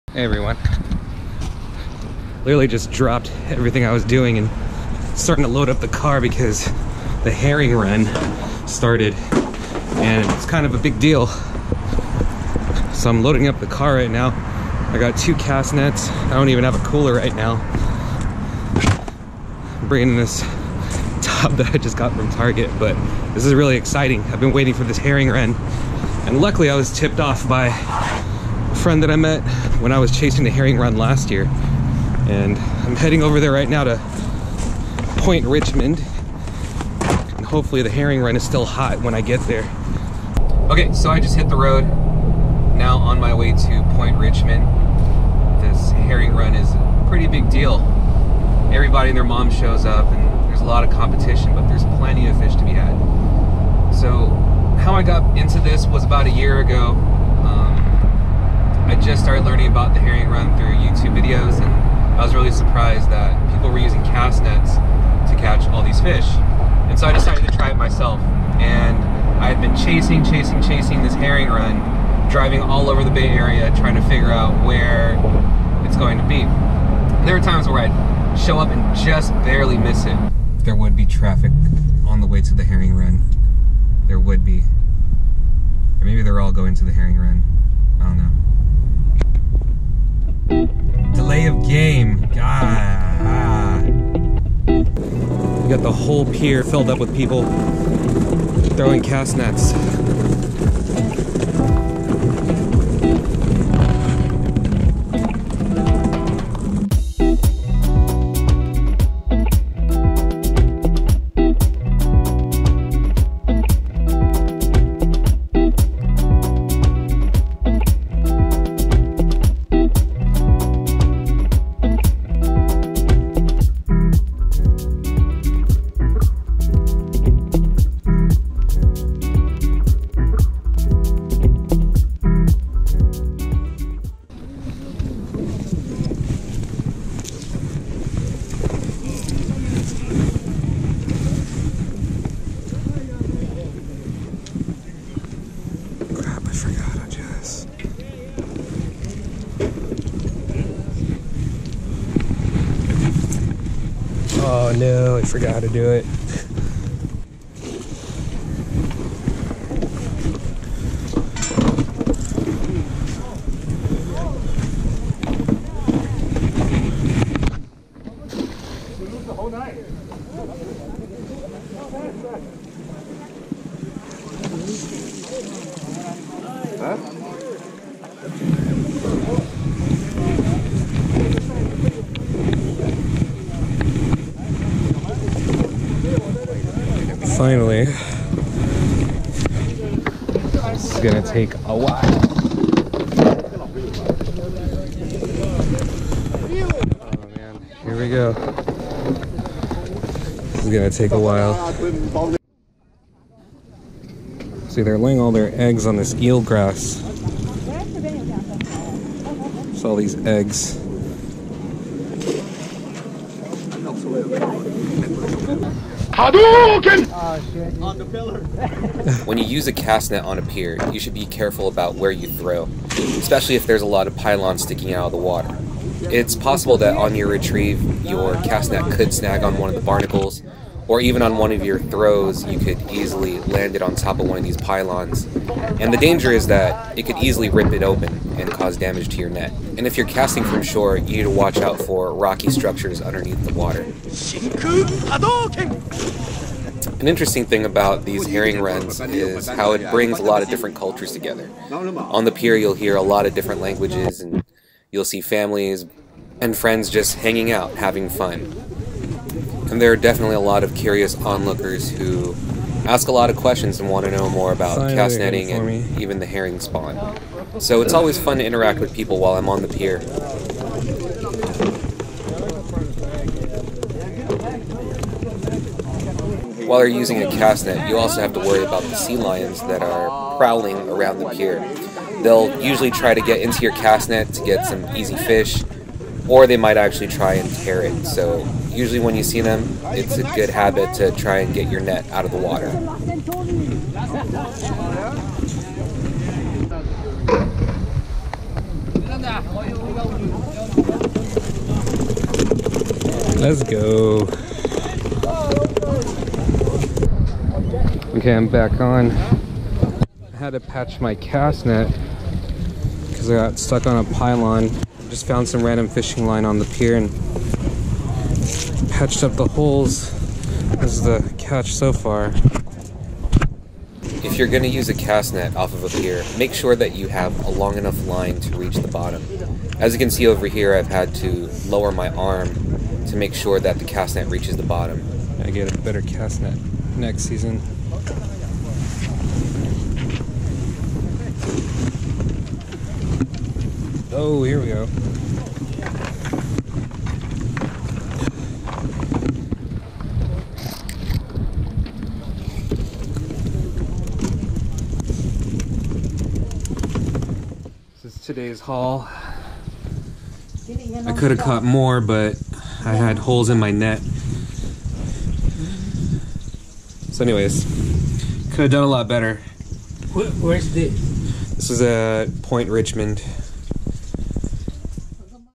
Chasing the Herring Spawn at Point Richmond (2019)
I received a text message from a friend that a herring spawn was happening at Point Richmond. I immediately packed my cast nets and went to Point Richmond to catch herring.

Filmed and Edited by Mark Aquino
Equipment: GoPro Hero 5 Black